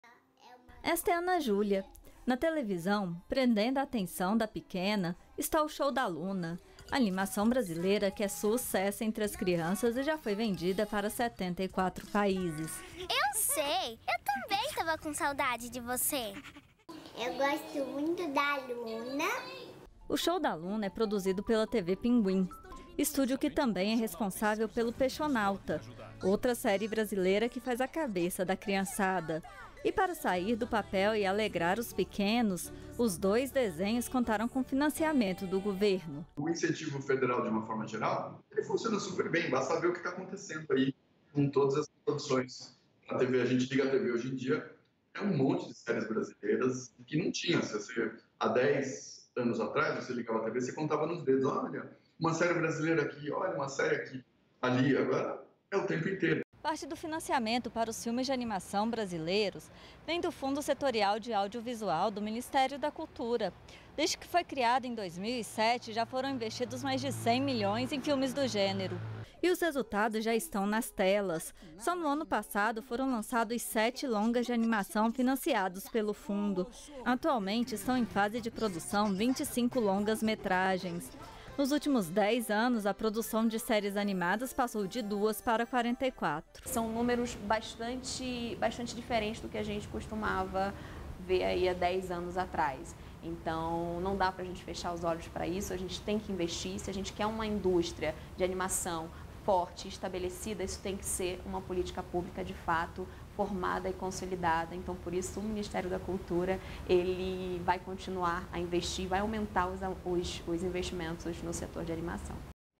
Animações brasileiras recebem incentivo do governo para sairem do papel
REPÓRTER NBR - 23.02.18: animações brasileiras como O Show da Luna e Peixonauta são sucesso entre as crianças. Ambos os desenhos são da TV Pinguim e contaram com financiamento do governo para sair do papel.